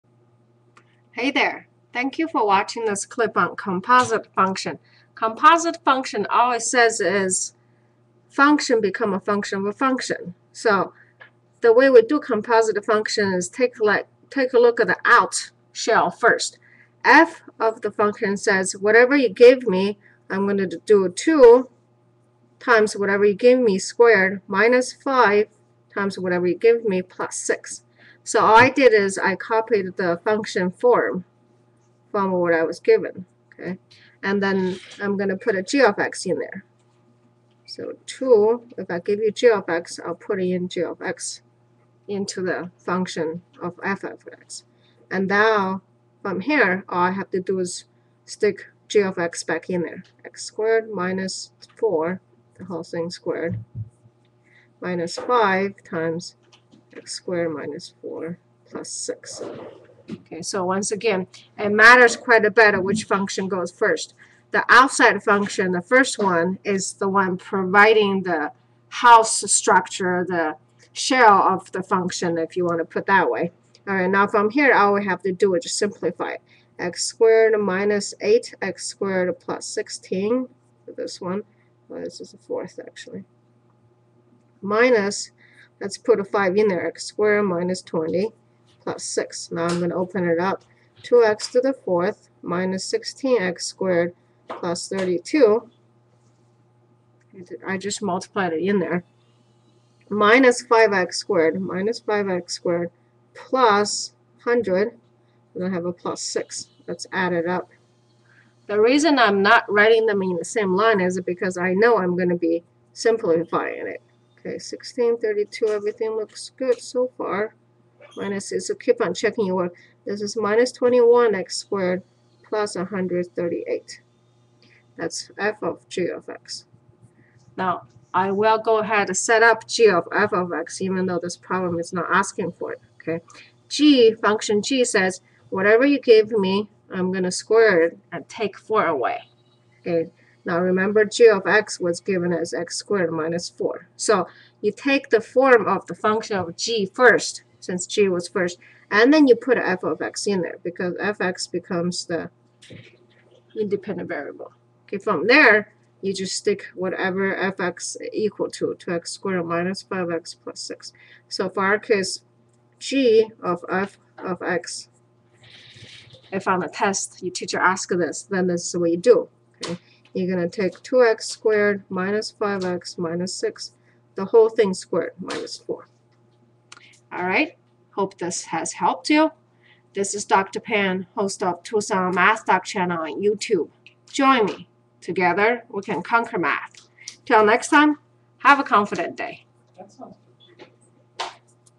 COMPOSITE FUNCTIONS EXPLAINED FAST | Tricky Example | Pre-Calculus
Dr. Pan solves the following problem: Find f ∘ g, when f(x) = 2x^2 - 5x + 6, and g(x) = x^2 - 4